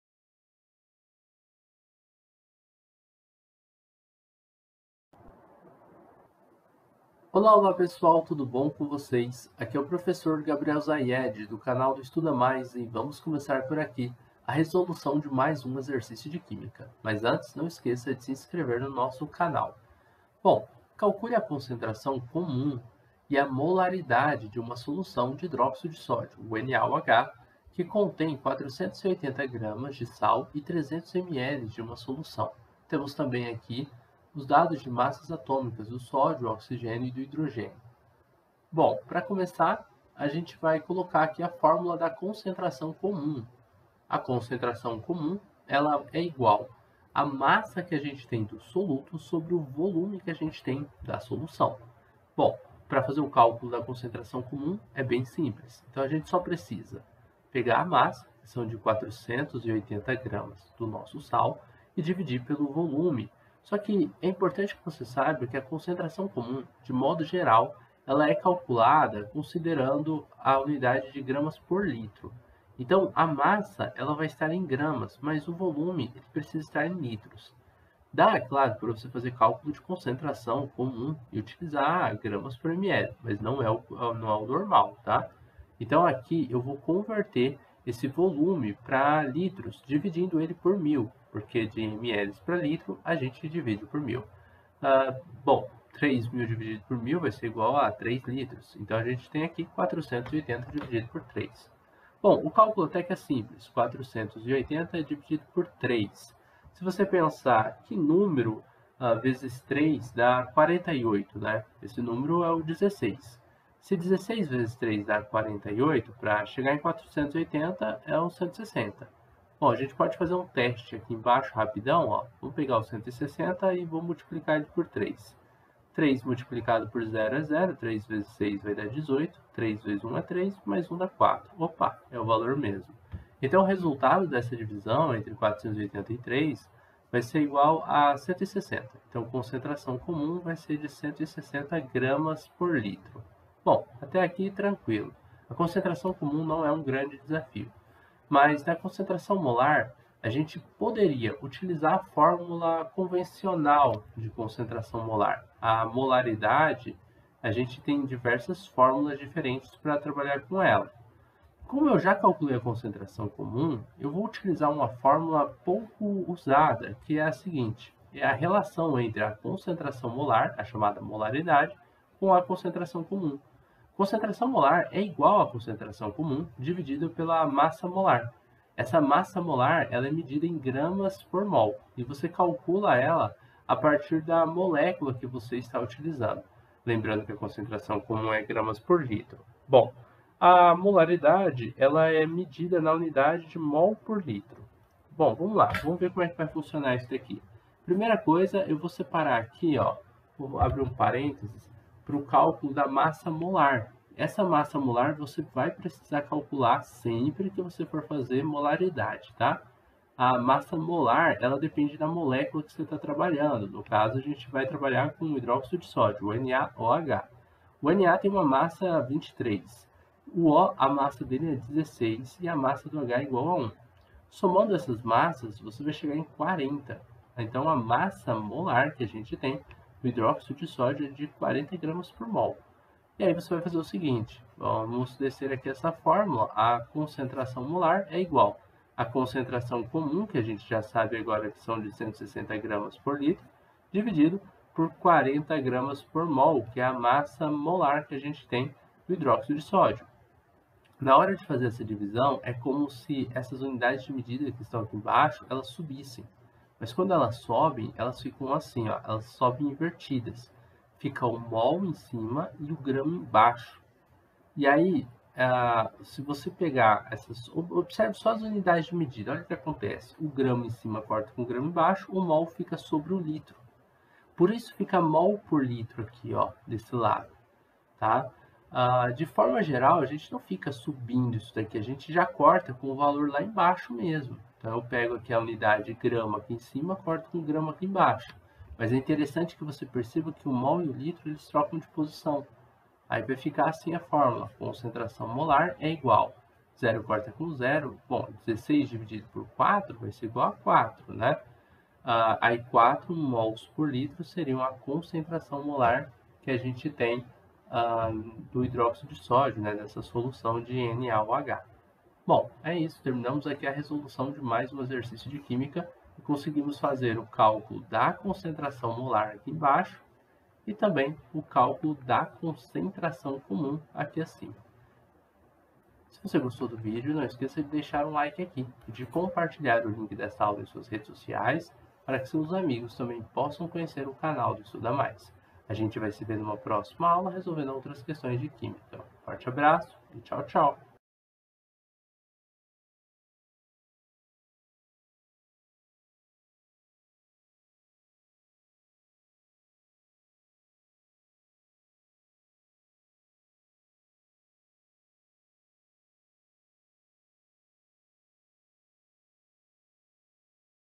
Calcule a concentração de uma solução de NaOH que contém 480g desse sal em 3000ml de solução.
Calcule a concentração comum e a molaridade de uma solução de NaOH que contém 480g desse sal em 3000ml de solução. ( Na=23; O=16; H=1 )